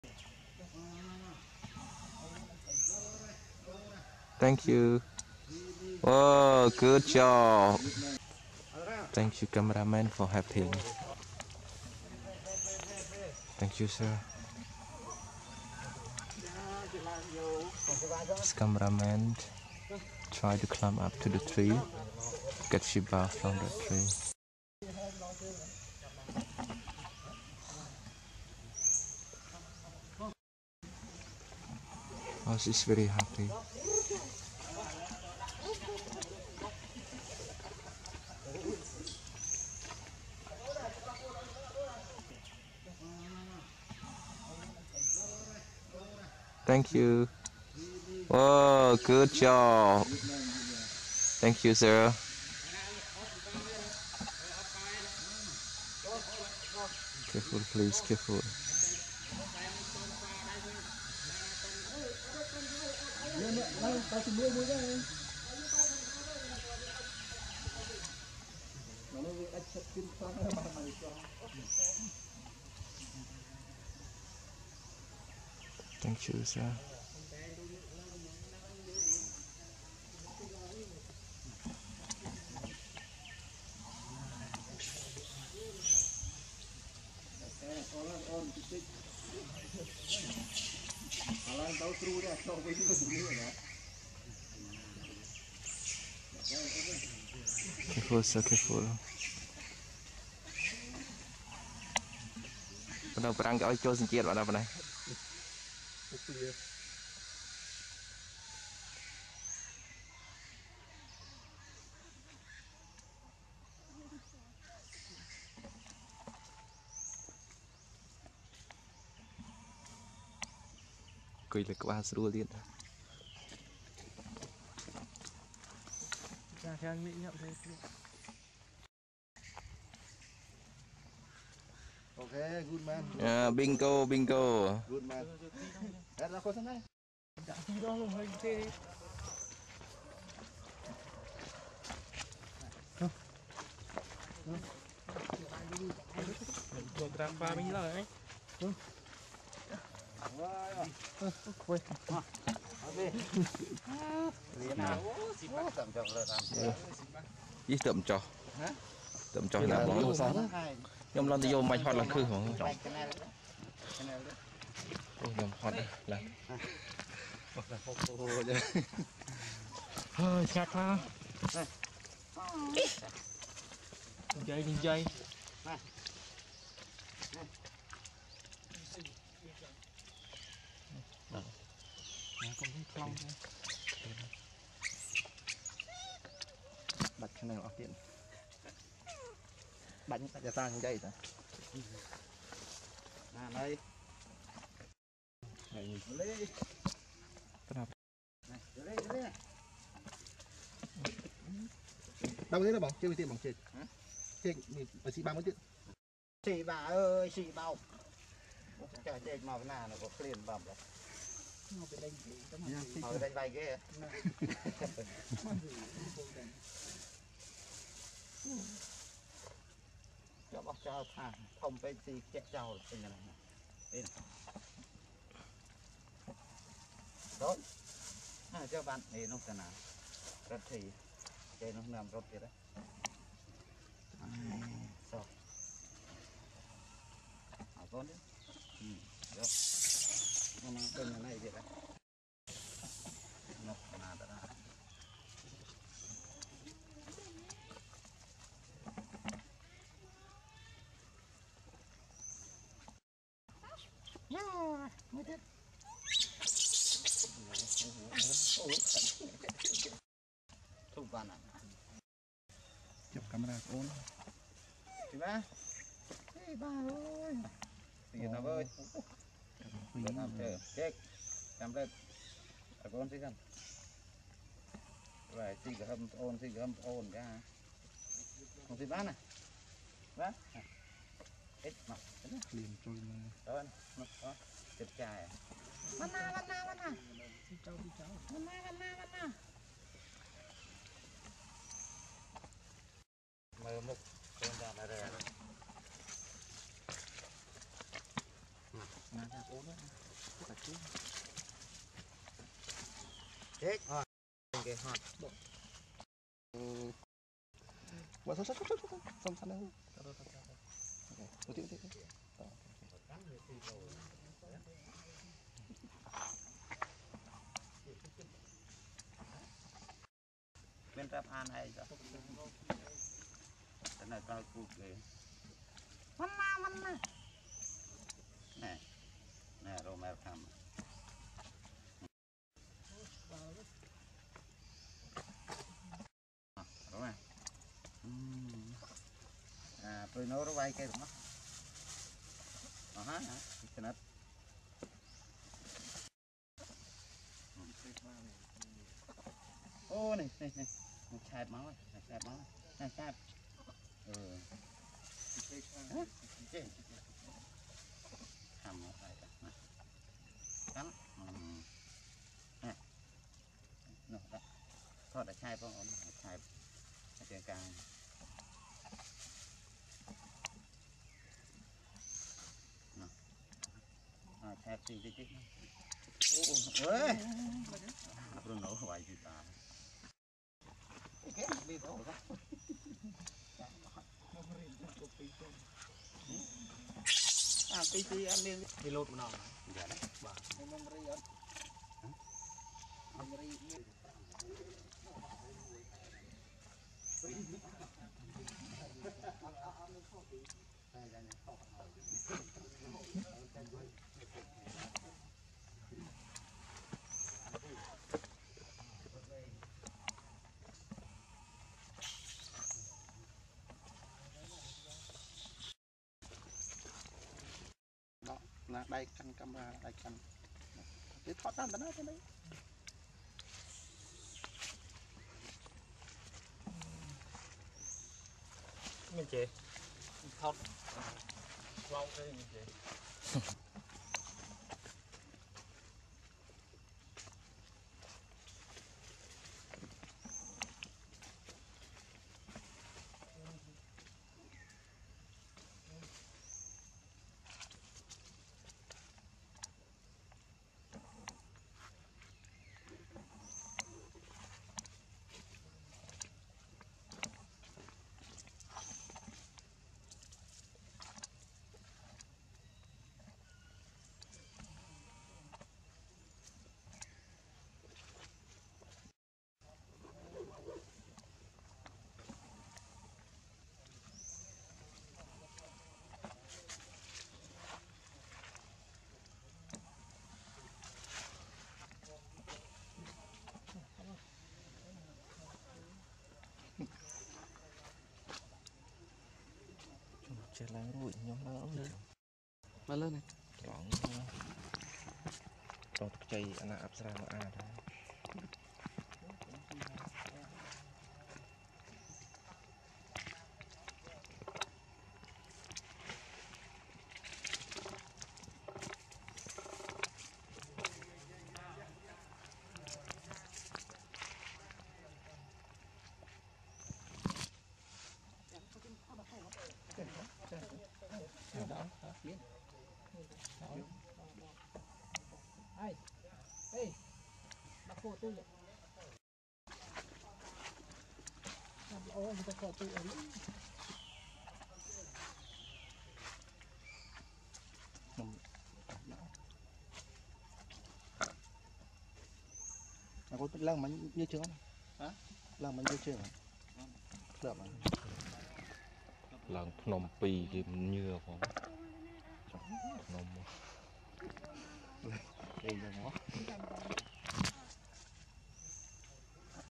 (17-12-21)Thanks God! Finally Cameraman Went Up To Take Poor Sheba From The Tree For Food.
I Will Show About: (17-12-21)Thanks God! Finally Cameraman Went Up To Take Poor Sheba From The Tree For Food.

Miss. Lina